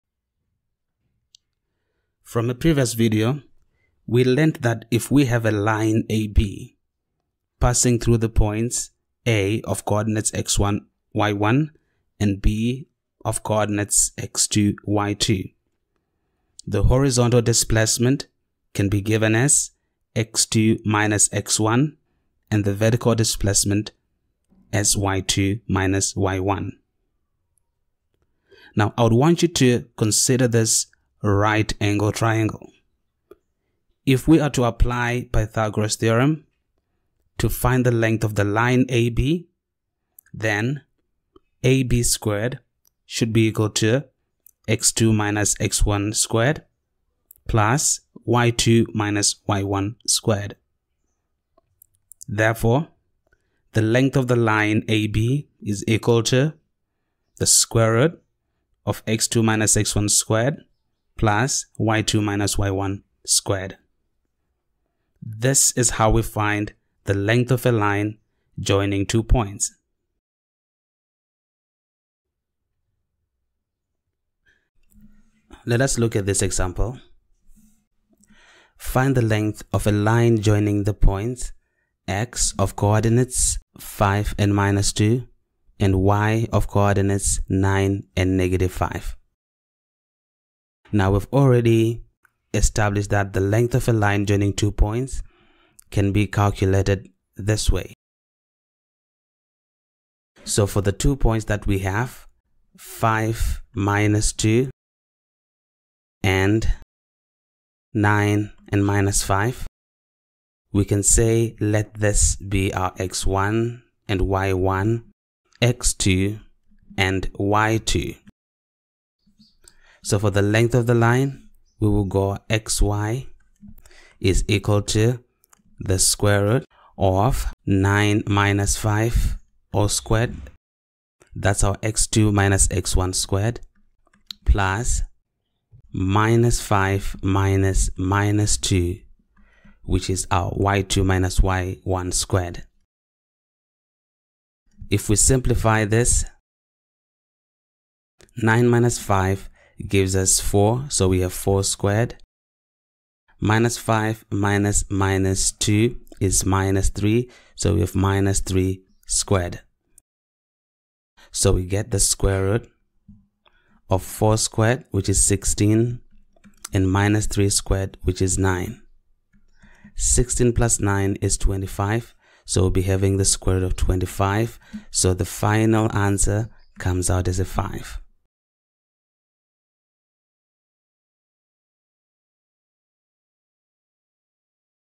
AS Maths: Length of a line joining two points
AS/ A Level Maths - Coordinate Geometry. Understand how to calculate the length of a line two points. Worked example included.